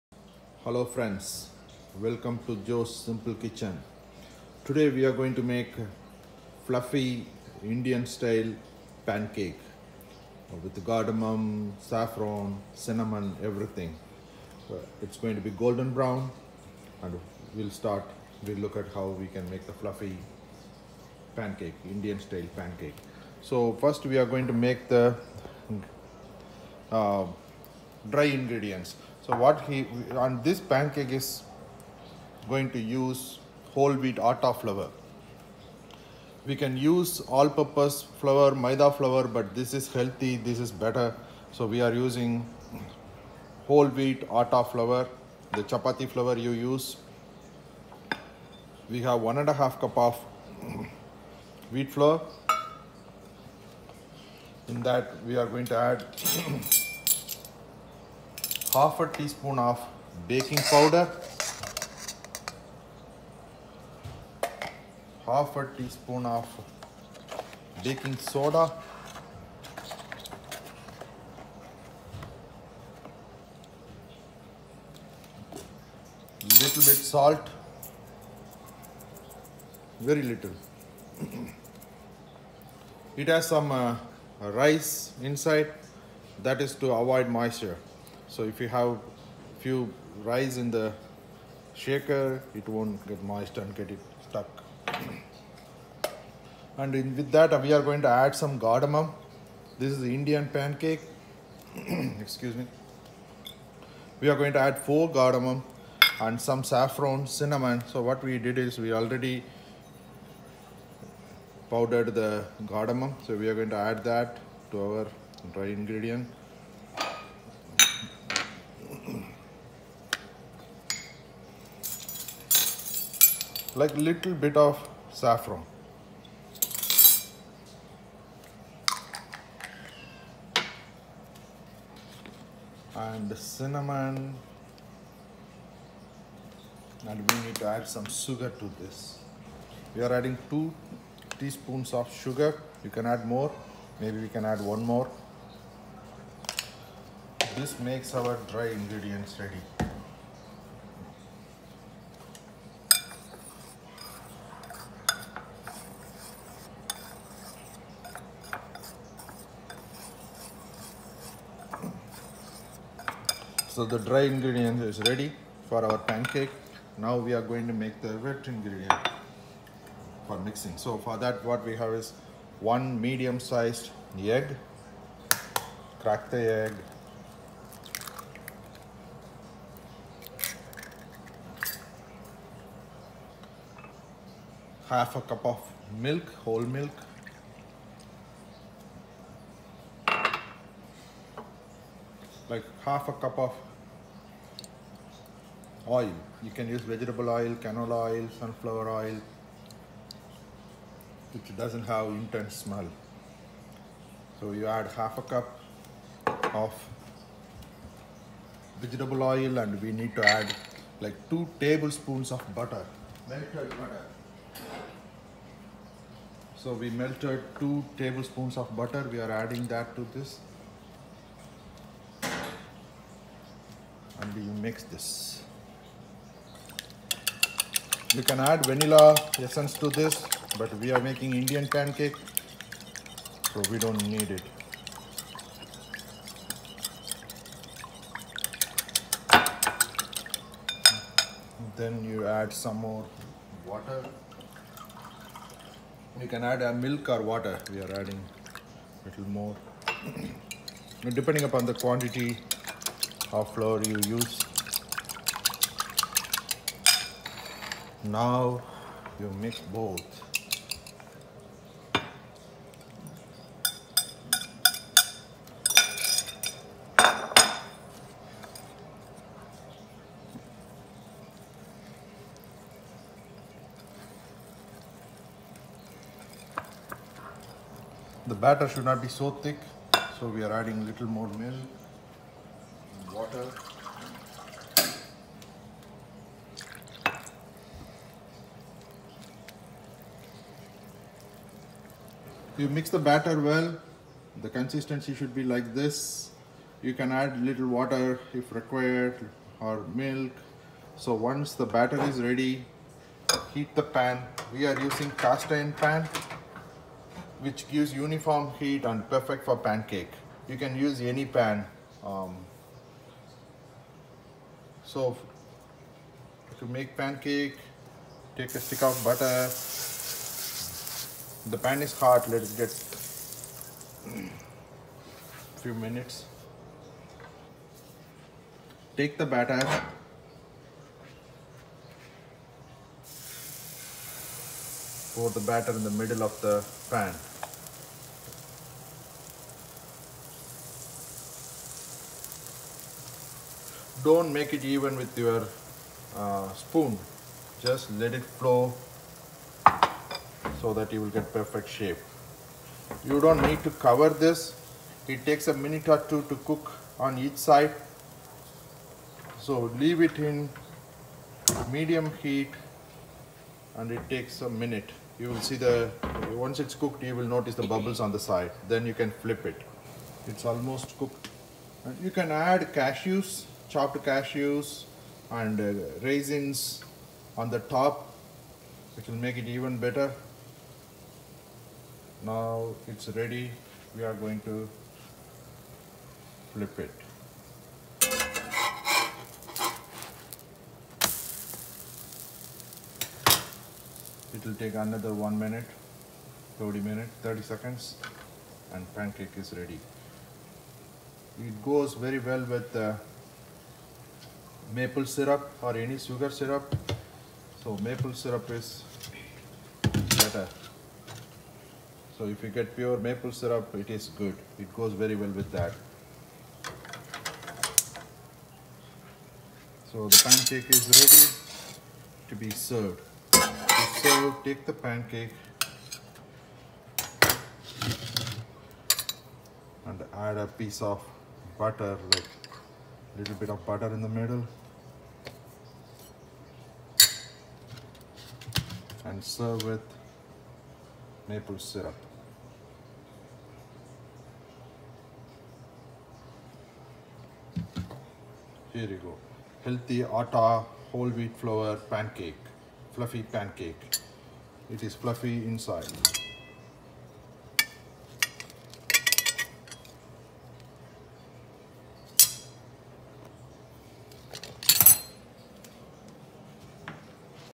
INDIAN PANCAKE | Whole Wheat Recipes| Indian Breakfast Recipes | Healthy Indian Recipes
Indian Pancake.

Healthy Whole Wheat Pancake. Elachi (cardamom) Saffron Pancake. How to make perfect Golden Brown Elachi Pancake using Whole Wheat flour.